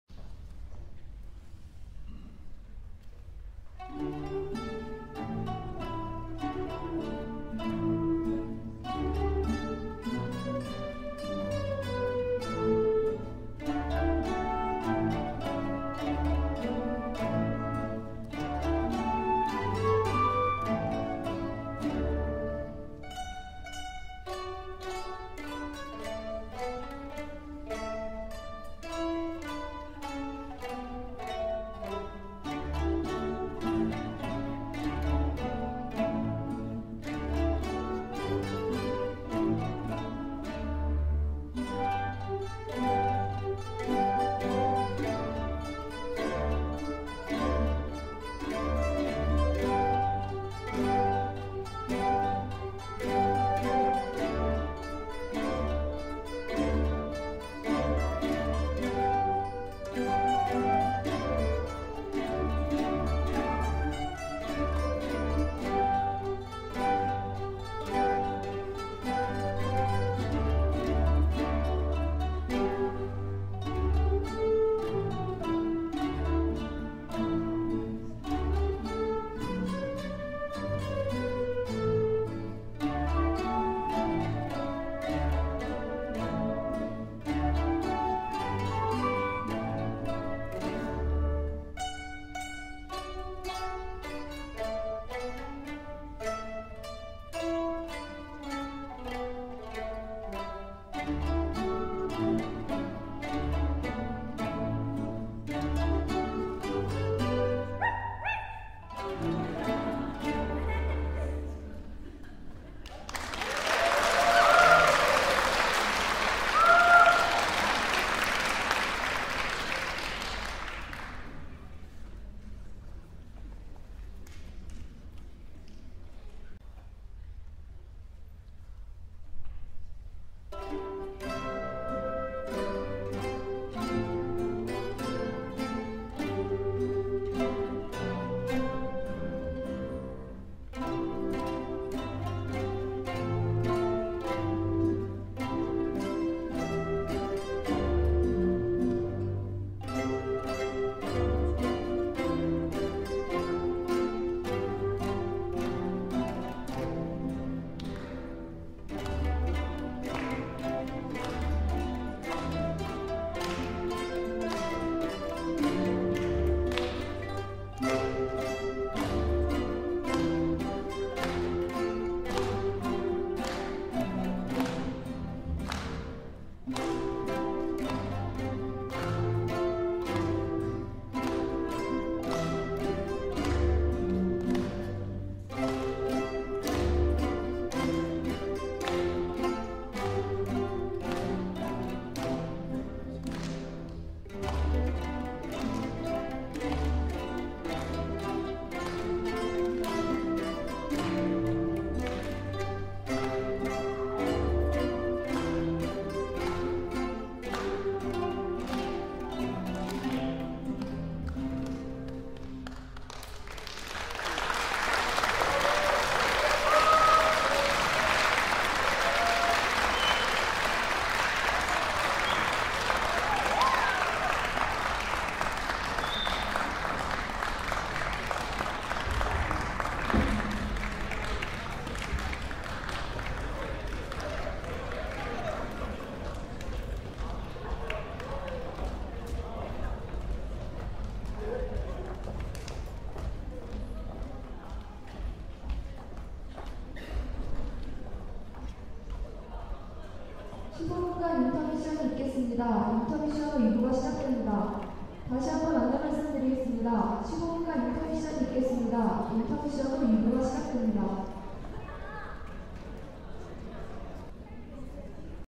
20110514 12 신입생연주
신입생 연주

2011/05/14
Spring Concert
Soongsil University Mandolin Orchestra

Conductor: TaeSoo Cho